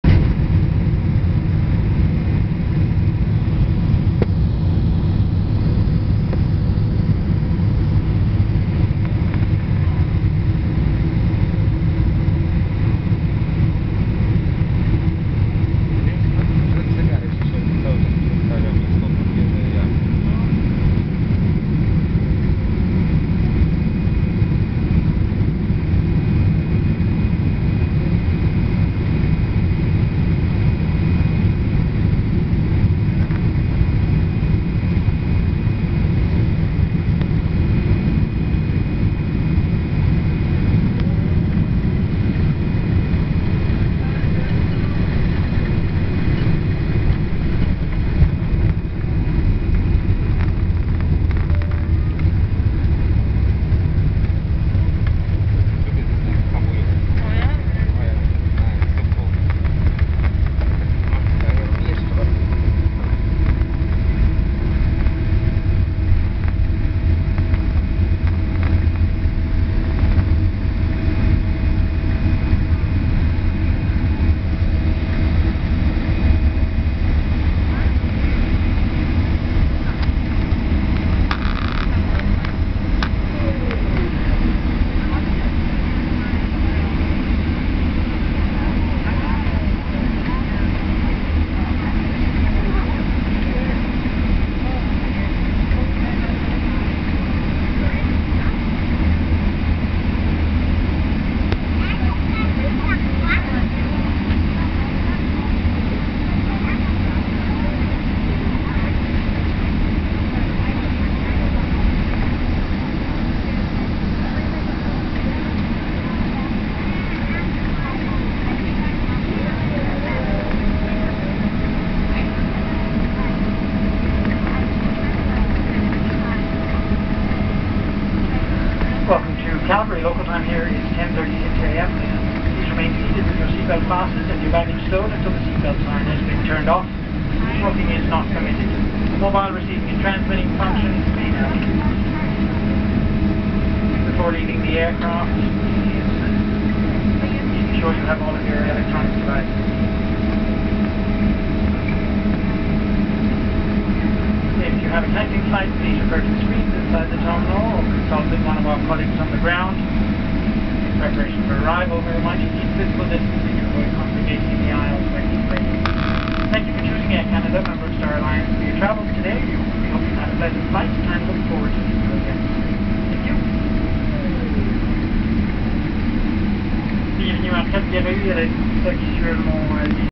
Air Canada Flight # 135 Part 2
AC135 with a Boeing 777-300ER on Landing at YYC on Runway 17L... Taken on 08/25/21